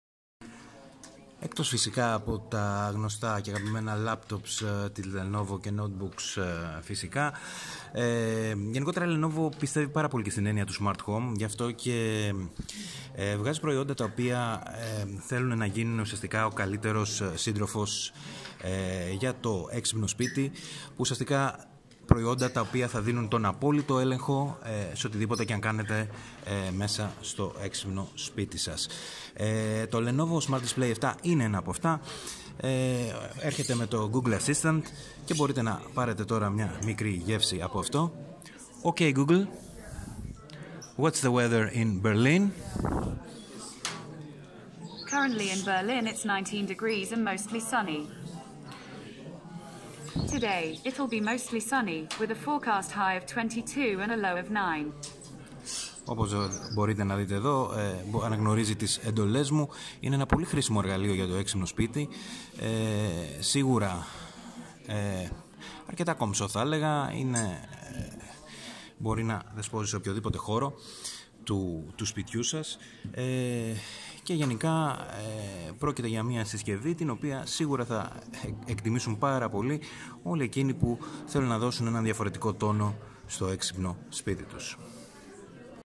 Lenovo Smart Display 7 Hands-On IFA 2019
To νέο Lenovo Smart Display φιλοδοξεί να φέρει ένα νέο αέρα στην διαχείριση του έξυπνου σπιτιού!